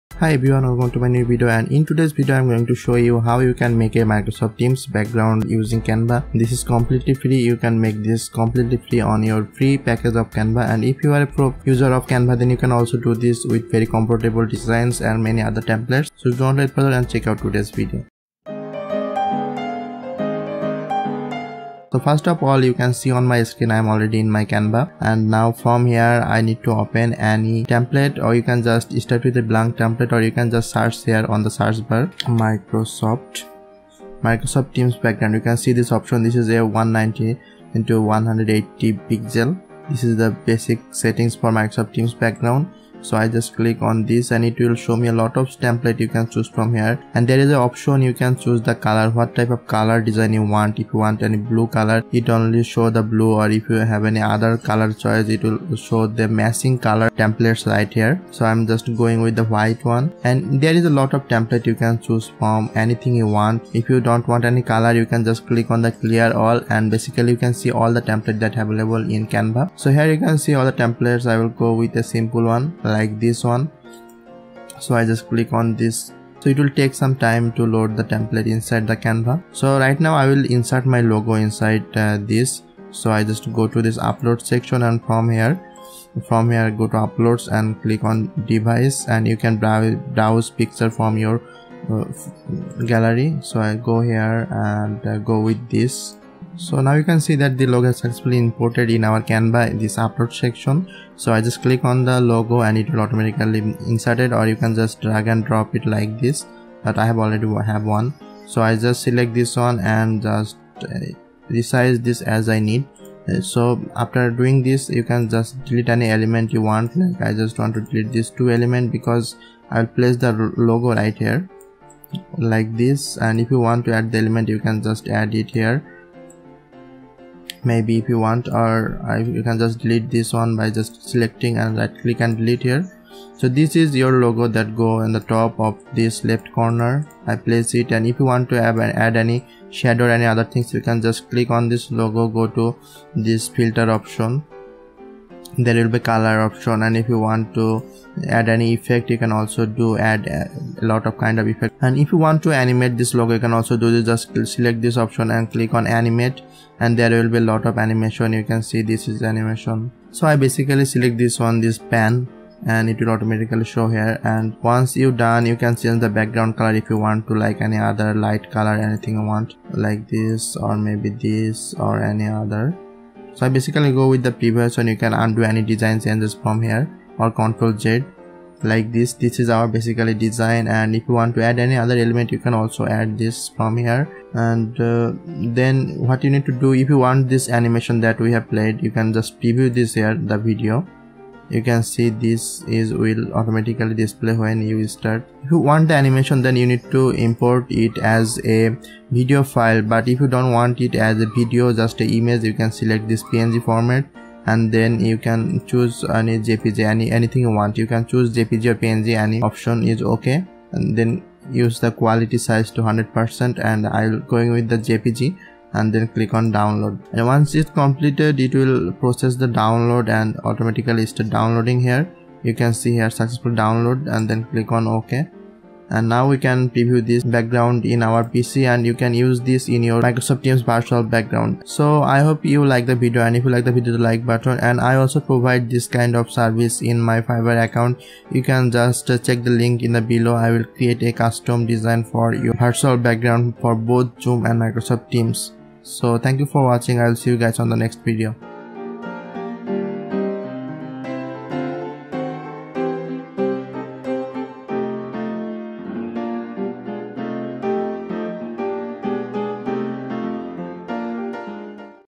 How to create Microsoft teams background in Canva with logo
In this video i show how you can make a custom virtual background with your logo completely free in canva online photo editor. There is a lots of free Microsoft teams background in canva that you can choose from.

Canva is a free photo editor. It allow to make Microsoft teams background template with logo. You can upload your company logo.